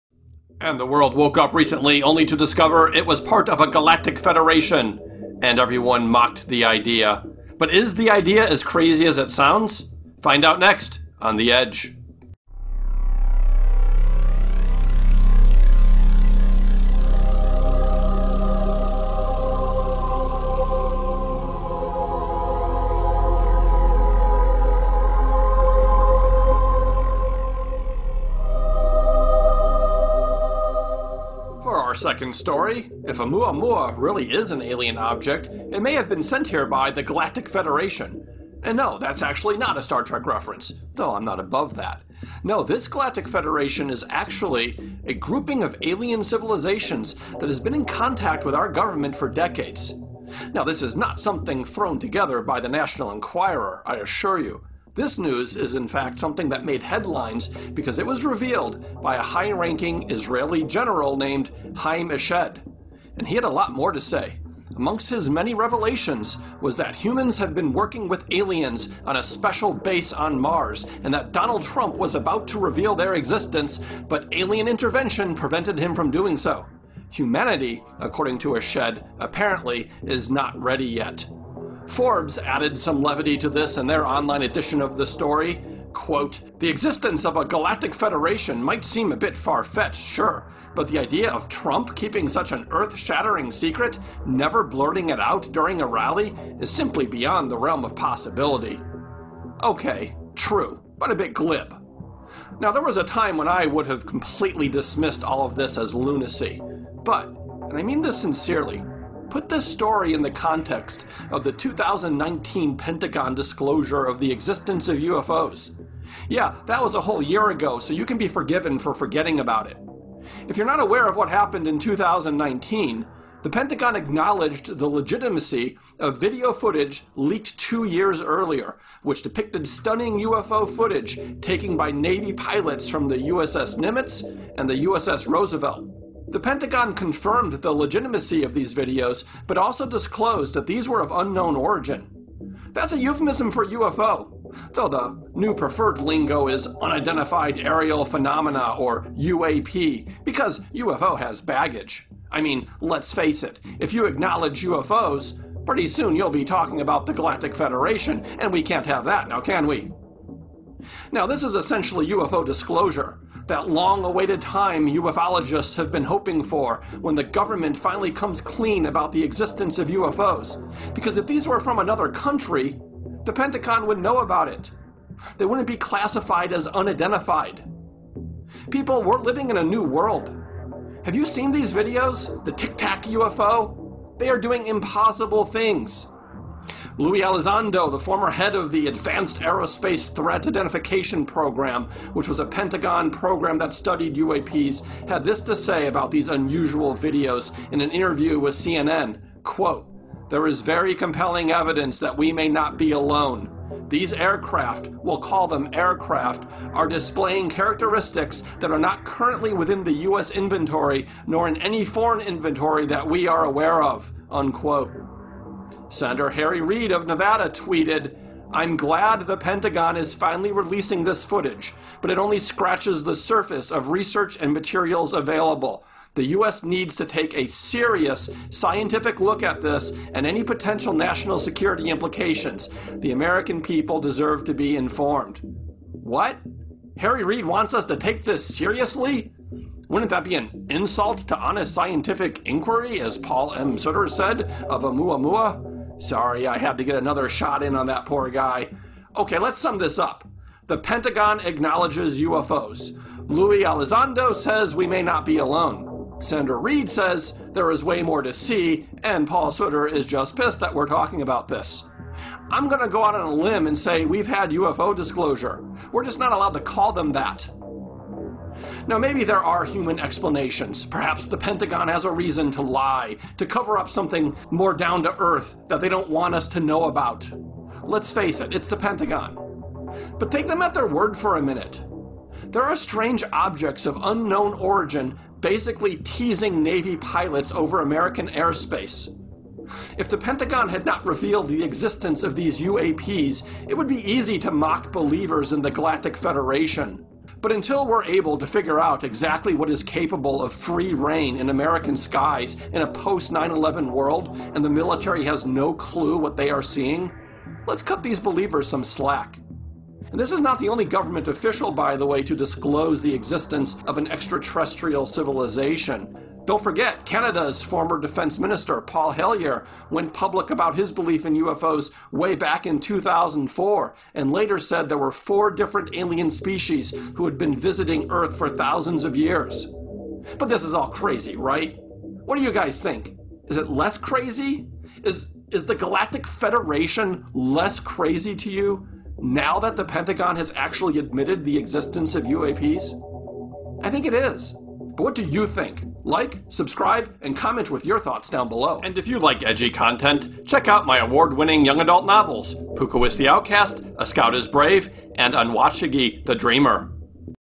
Is the Galactic Federation Really all that Crazy?
Mainstream news outlets ran with the revelation from Israeli General, Haim Eshed, that Americans have been secretly working with aliens on Mars, aliens who in turn are part of a Galactic Federation. The idea might very well be less crazy than it sounds!

Story 2 – The Galactic Federation
Adela Suliman and Paul Goldman “Former Israeli Space Security Chief Says Extraterrestrials Exist, and Trump Knows About It,” Dec. 8, 2020.
Dani Di Placido Senior Contributor. “2020 Looks To Be Ending With A Bang, With The Reveal Of The ‘Galactic Federation,’” December 8, 2020.
Gimbal_The_First_Official_UAP_Footage_from_the_USG_for_Public_Release.webm. Public Domain. No attribution required.